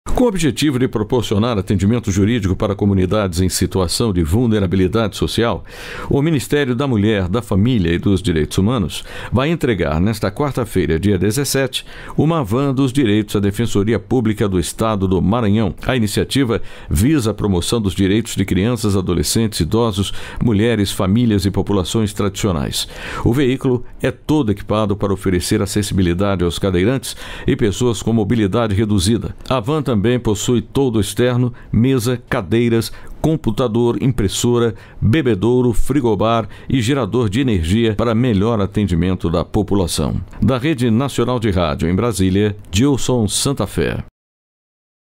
17/11/2021 - MARANHÃO RECEBE VAN DOS DIREITOS À DEFENSORIA PÚBLICA
Objetivo do Ministério da Mulher, da Família e dos Direitos Humanos é proporcionar atendimento jurídico para comunidades em situação de vulnerabilidade social

Com o objetivo de proporcionar atendimento jurídico para comunidades em situação de vulnerabilidade social, o Ministério da Mulher, da Família e dos Direitos Humanos vai entregar nesta  quarta-feira (17), uma Van dos Direitos à Defensoria Pública do Estado do Maranhão.

A iniciativa visa a promoção dos direitos de crianças, adolescentes, idosos, mulheres, famílias e populações tradicionais.

O veículo  é toda equipado  para oferecer acessibilidade aos cadeirantes e pessoas com mobilidade reduzida. A Van  também possui toldo externo, mesa, cadeiras, computador, impressora, bebedouro, frigobar e gerador de energia para melhor atendimento da população.

registrado em